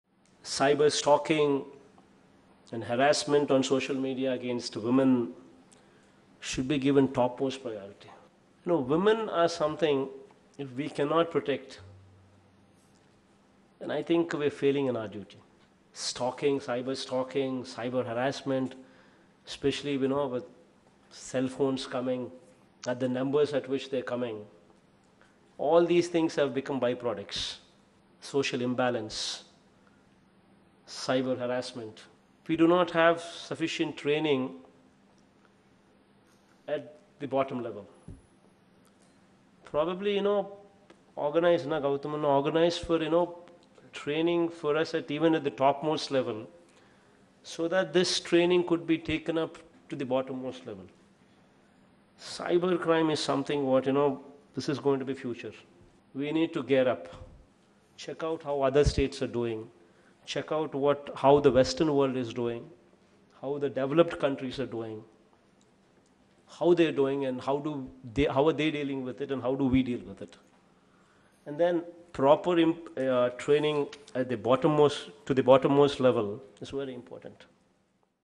AP CM YS Jagan on cyber crime at Collectors Conference at Vijayawada.
AP CM YS Jagan on cyber crime at Collectors Conference at Vijayawada.-25th june  2019